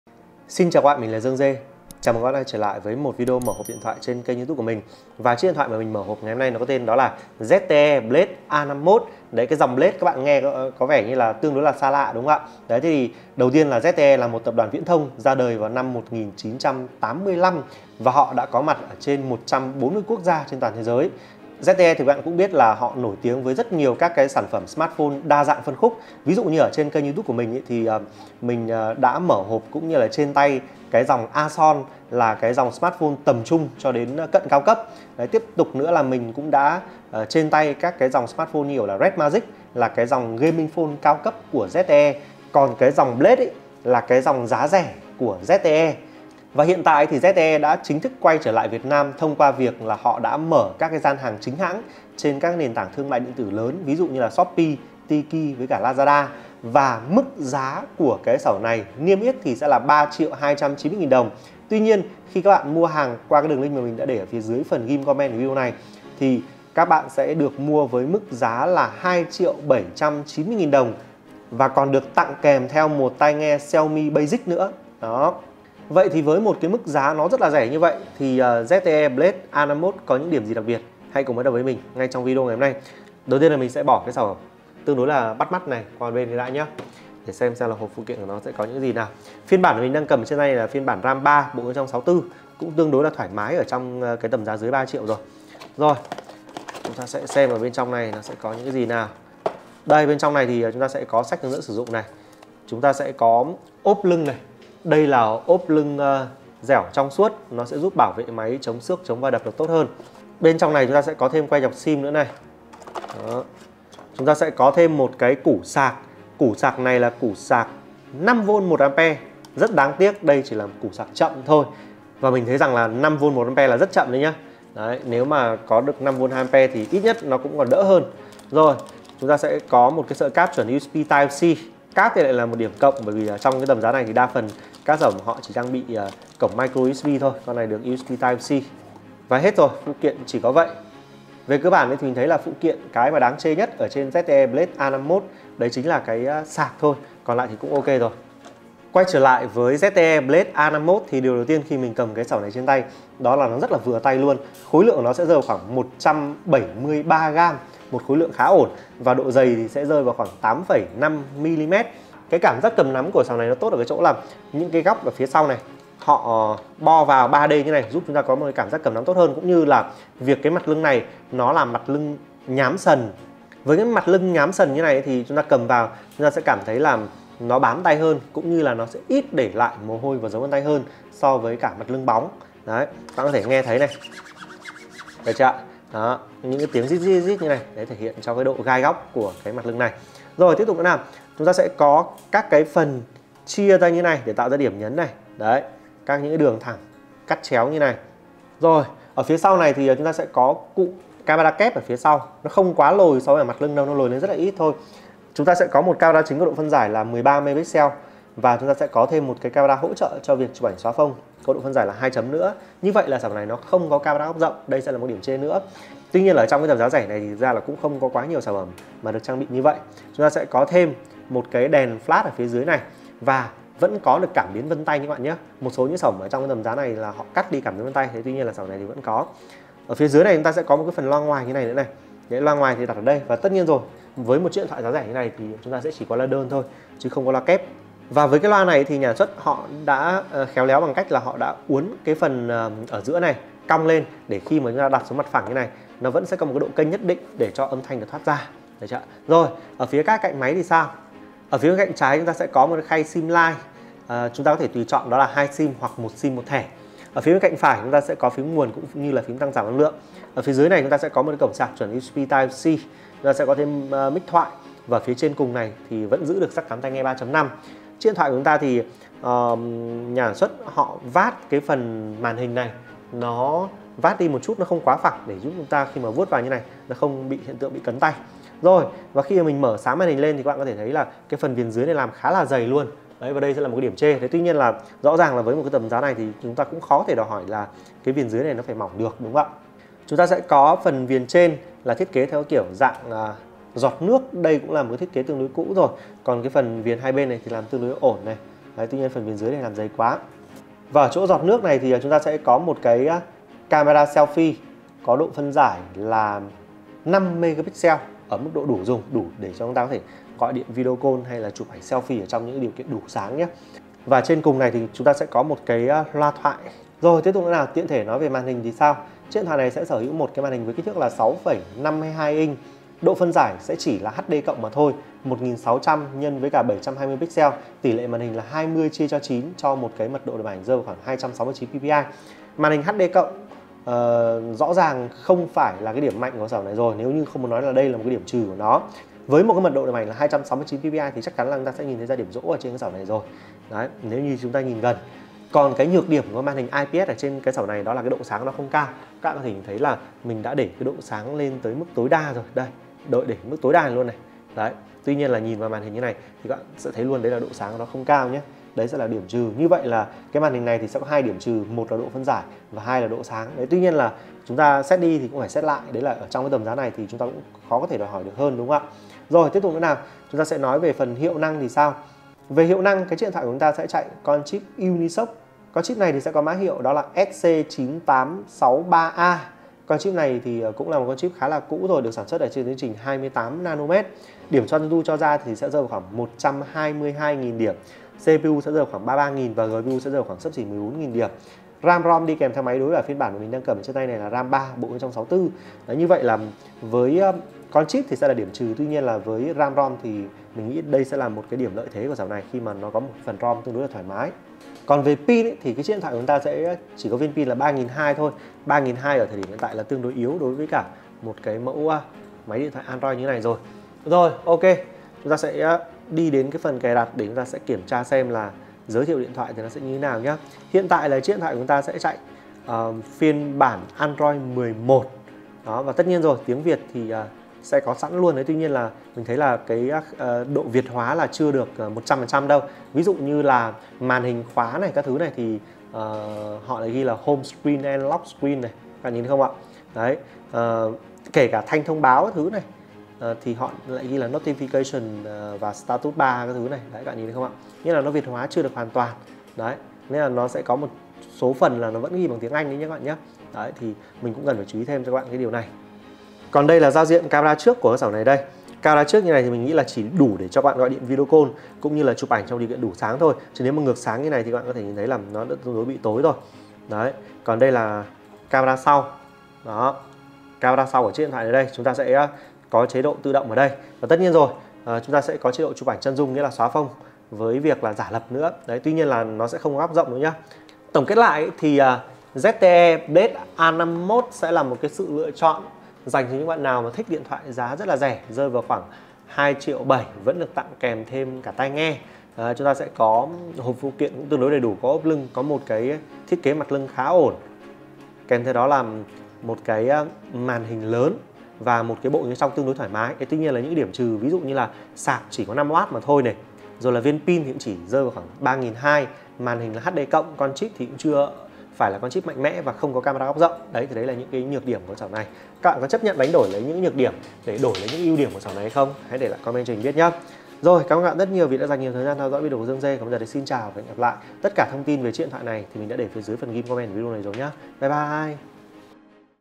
Mở hộp ZTE Blade A51 SIÊU RẺ 2,8 TRIỆU sẽ có gì?
Dương Dê Reviewer chuyên đánh giá các sản phẩm điện thoại và đồ công nghệ